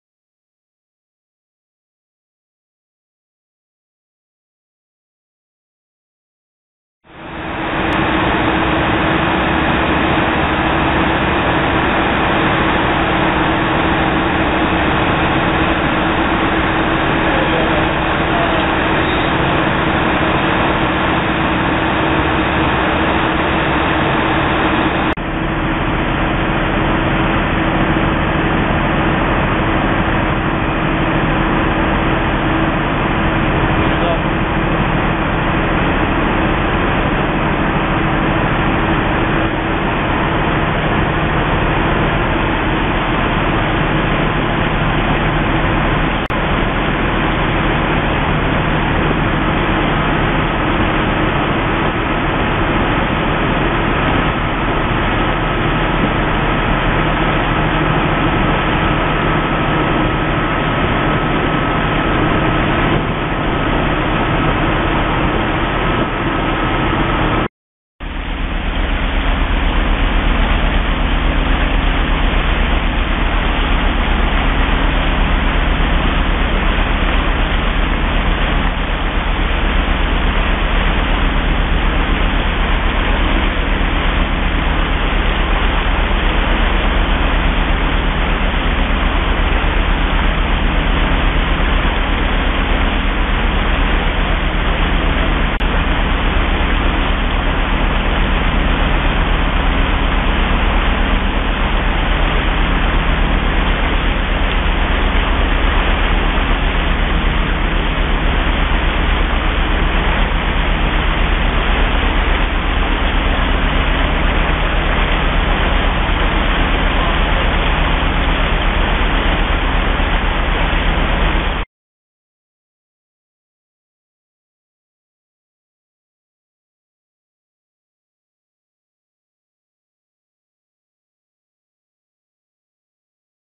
Oceanscience UnderwayCTD on a Hydrographic Survey in Chile with Fugro Pelagos
The UnderwayCTD is used to gather 300m CTD profiles at 4-5kts on a
survey in Chile.